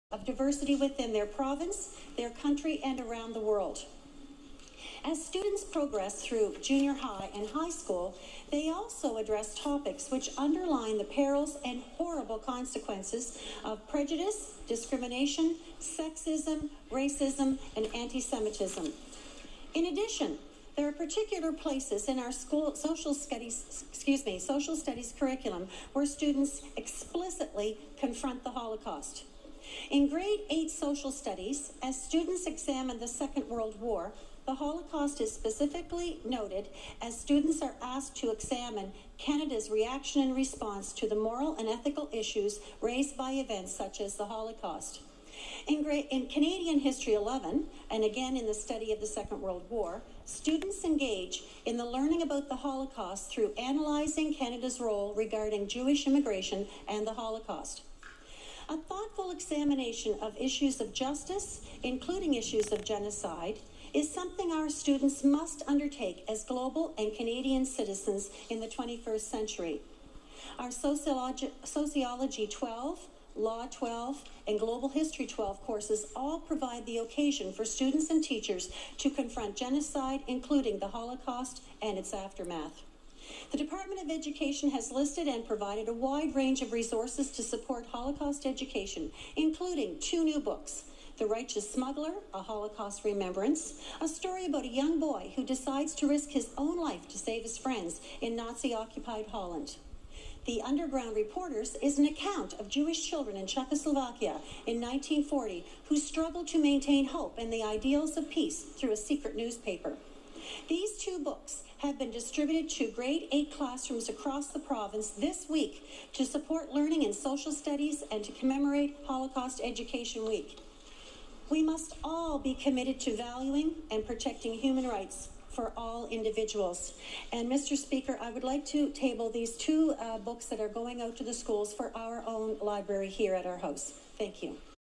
Hon. Ramona Jennex in Halifax
Ramon Jennex in Halifax on November 1, 2011.  Statement by Minister of Education.